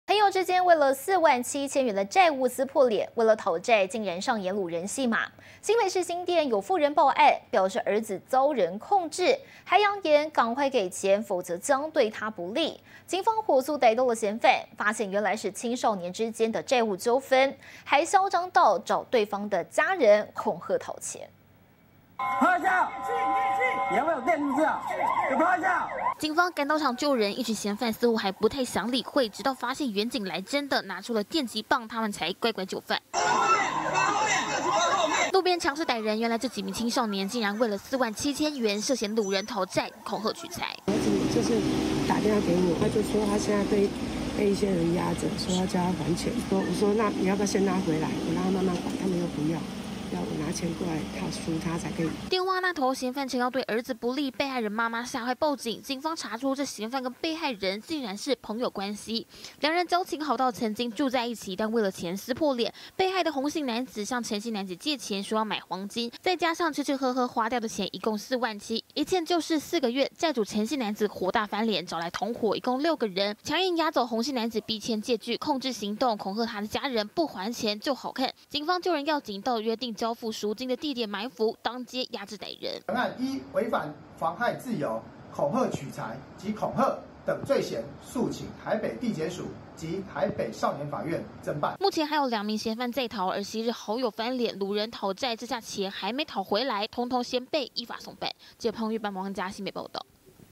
兒遭人控制！母嚇壞報案 警埋伏壓制逮3人
朋友間為了四萬七千元債務撕破臉，為了討債竟然上演擄人戲碼。新北市新店有婦人報案，表示兒子遭人控制，揚言趕快給錢，否則將對他不利，警方火速逮到嫌犯，發現原來是青少年之間債務糾紛，囂張到找對方家人恐嚇討錢。